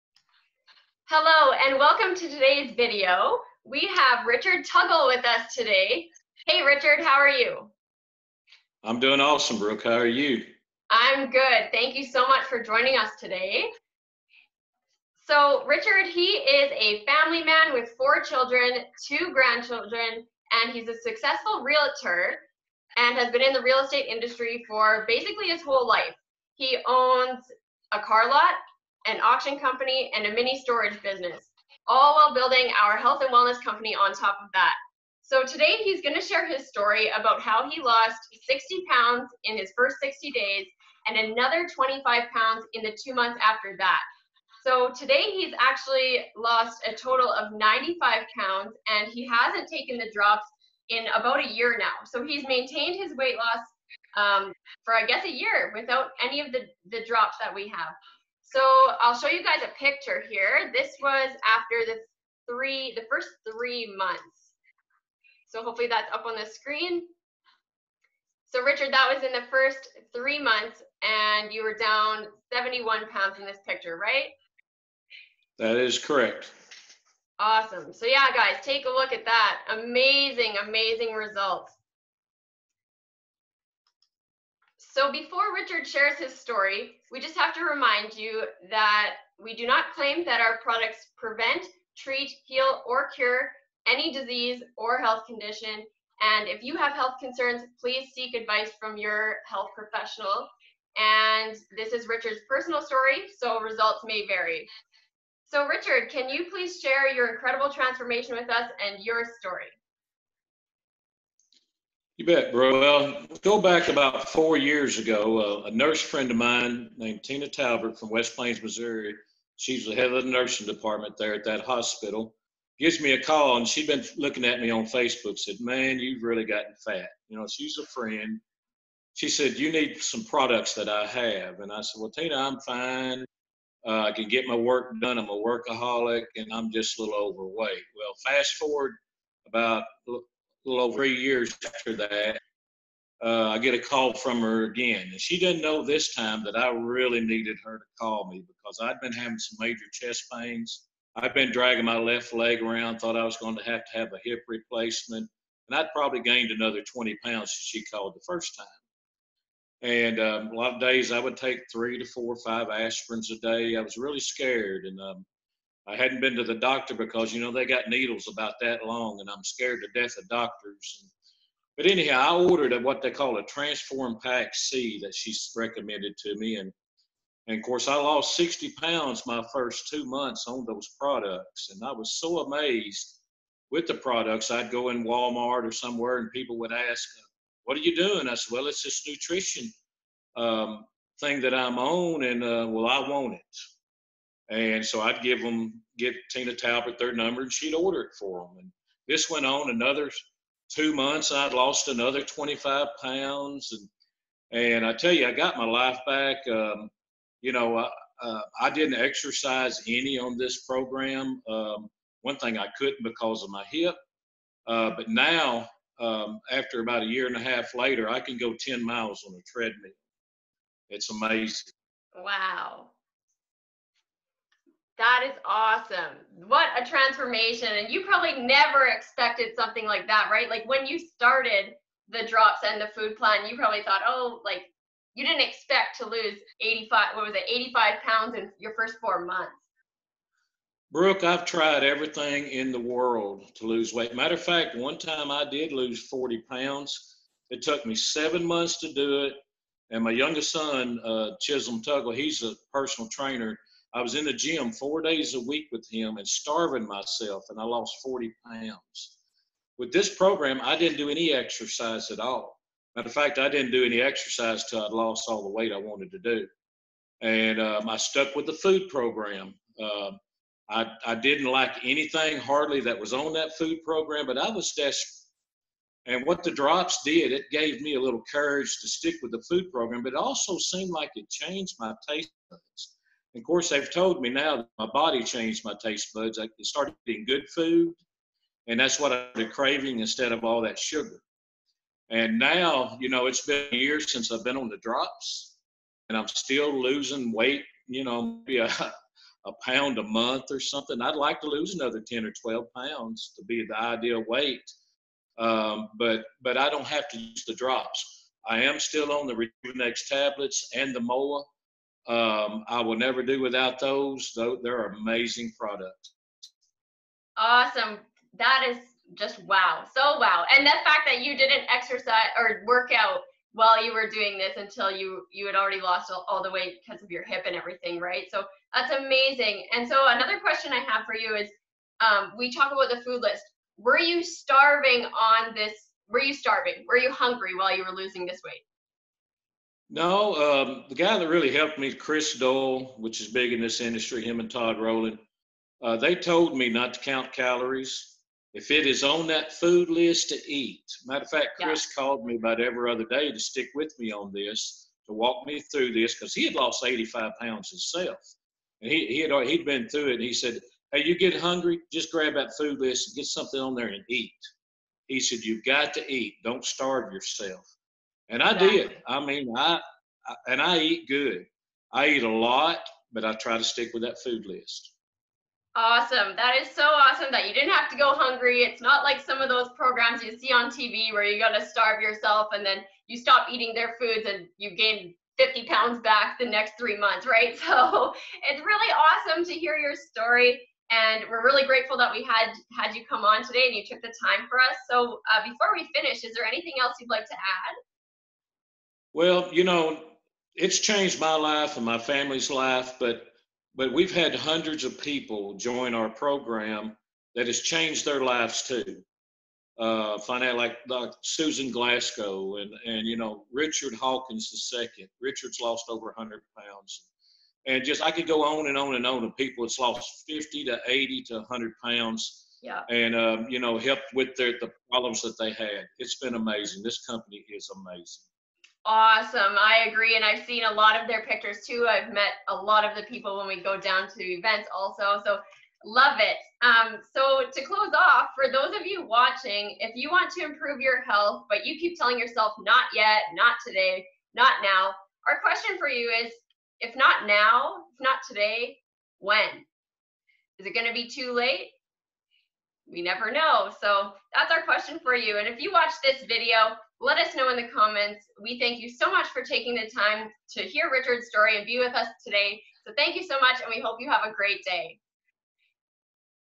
❤ Health & Wellness Talk Show | Richard Tuggle
📣"You've really gotten FAT!" 😱 That's what Richard's friend said to him!

👀 WATCH Richard Tuggle's story about how he lost 85lbs in 4 months! 👏